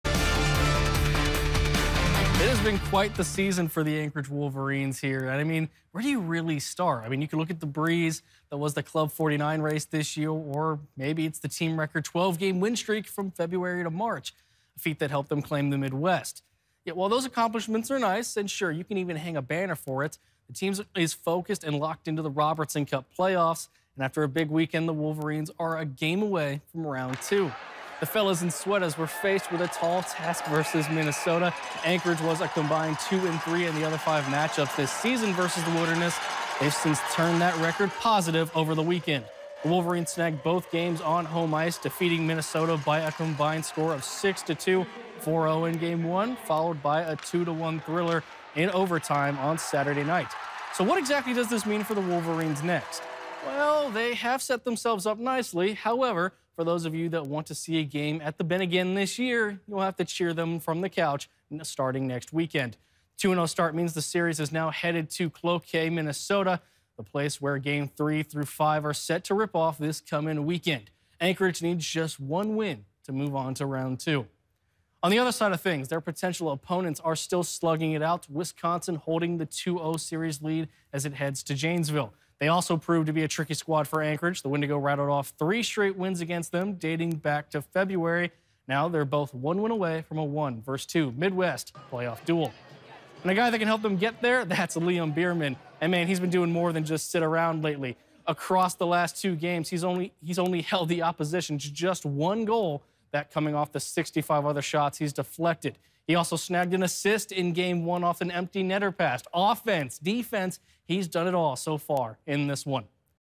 Anchorage Wolverines look to sweep opening round series of NAHL Playoffs
The Anchorage Wolverines defeated the Minnesota Wilderness twice over the weekend to take a commanding 2-0 series lead.